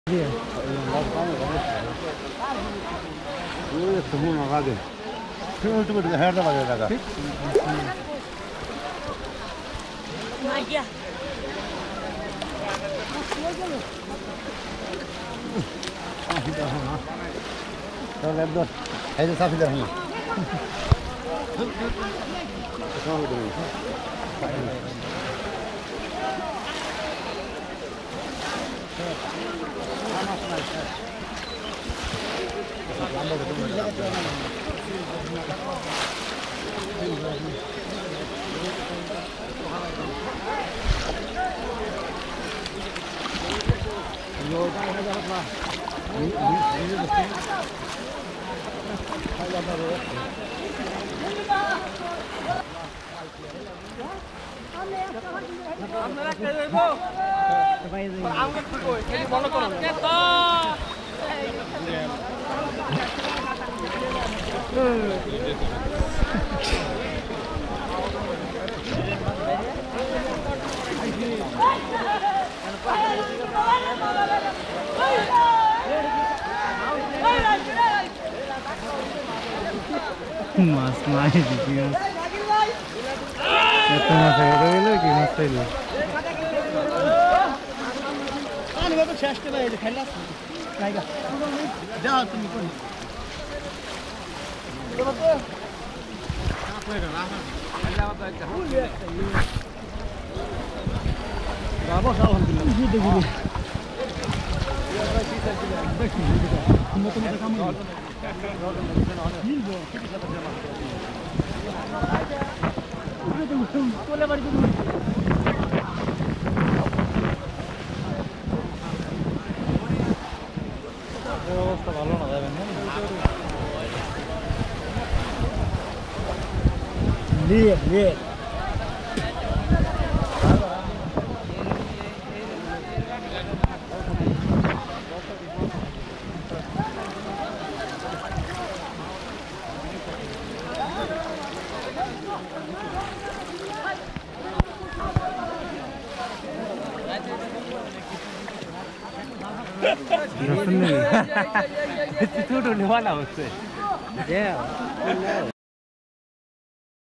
Big Fish Catching | Village Fisherman Great Fishing
Village traditional polo fishing and best festival fishing.